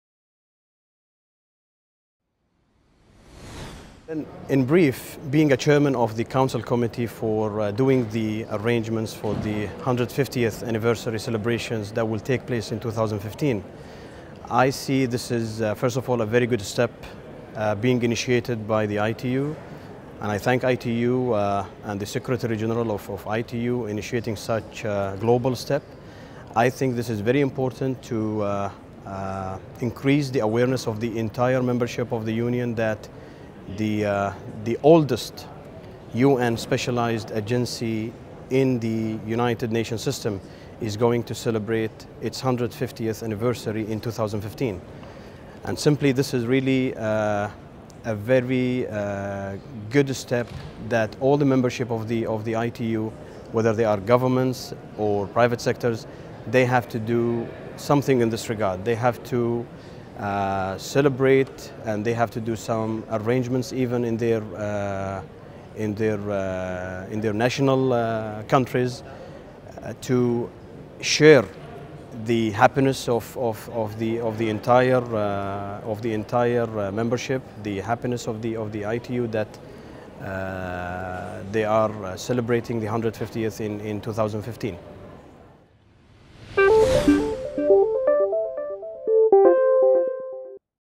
ITU 150 SOUNDBYTE: Nasser A. Bin Hammad, Senior Manager, UAE Telecommunication Regulatory Authority
'ITU is celebrating its 150th Anniversary in 2015.
In your opinion, what is one of its most important contributions?'

Soundbites on the upcoming 150th Anniversary of ITU:
Nasser A  Bin Hammad, Senior Manager, Internal Affairs, UAE Telecommunication Regulatory Authority, UAE